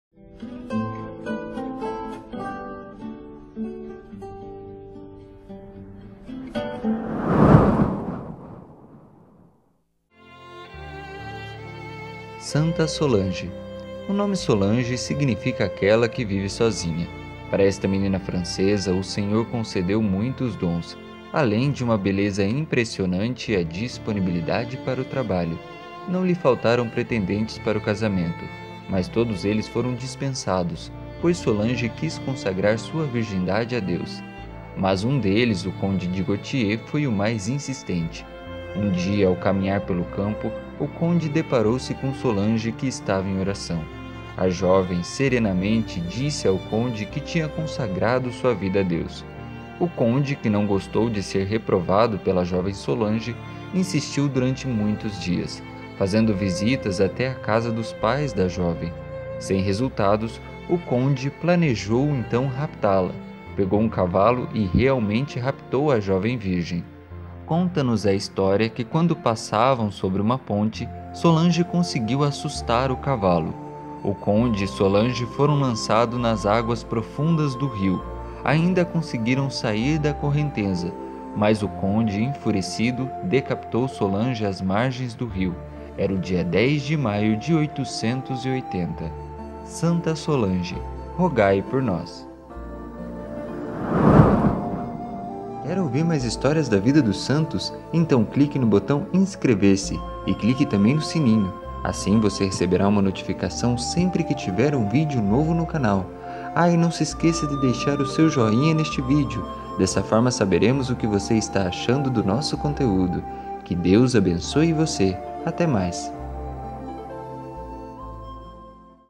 Santa Solange - 10/05 | Santo do Dia
No Santo do Dia de hoje celebramos a memória de Santa Solange, virgem e mártir que, por sua fidelidade a Jesus, Divino Esposo. recebeu a coroa do mártírio.